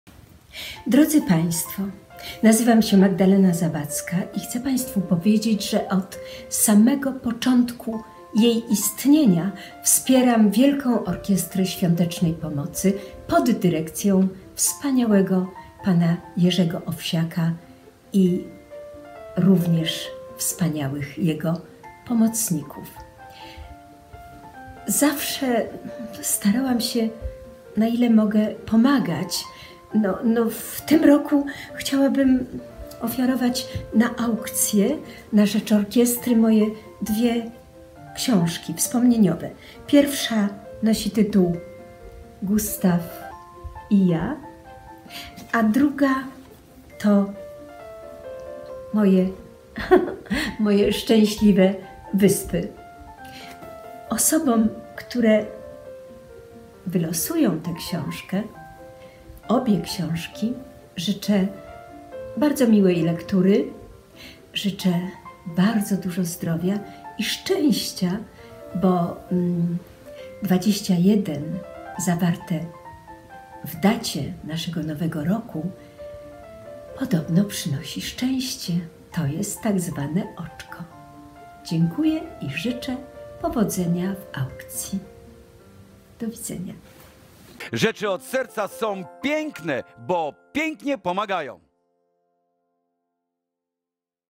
Książki Magdaleny Zawadzkiej z autografami
Wylicytuj egzemplarze książek Magdaleny Zawadzkiej z jej autografami!
"Gustaw i ja" - przepiękna historia miłości Magdaleny Zawadzkiej i Gustawa Holoubka przedstawiona z niezwykłą wrażliwością i poczuciem humoru właściwym autorce opowieści. Egzemplarz z autografem autorki.
"Moje szczęśliwe wyspy"- opowieści o przyblokowym podwórku i dziecięcych zabawach, wspomnienia codziennego życia, dawnego smaku chleba, truskawek i wody z saturatora, anegdoty związane z ludźmi i sprawami, do których po latach, tak jak autorka, lubimy powracać – jak do bezpiecznej przystani.

Gratka dla Łowców Autografów!